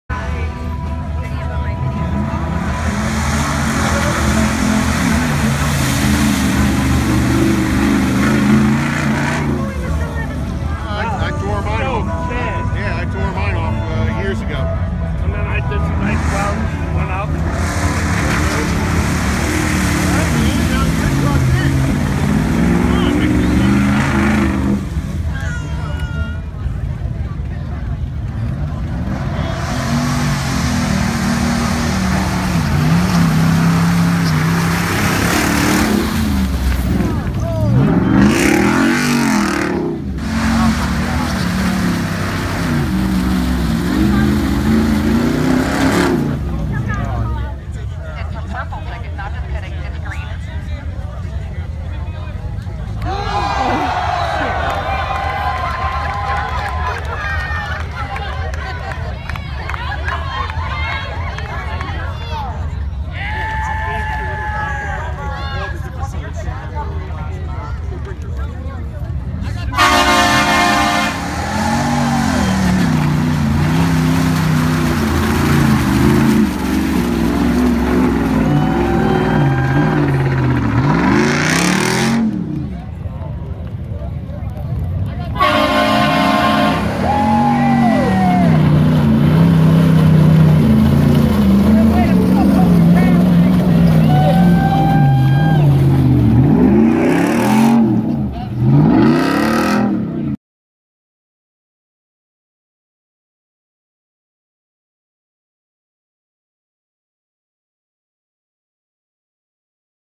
2017 Ram Power Wagon Off-Road Extreme 4x4 Mud Bogging - Most Capable Off-Road Pickup Available
2017 Power Wagon Extreme 4x4 Off-Roading in Mud.
Northeast 4x4 Off Road Club ( NJ NY PA MD CT RI VT NH MA )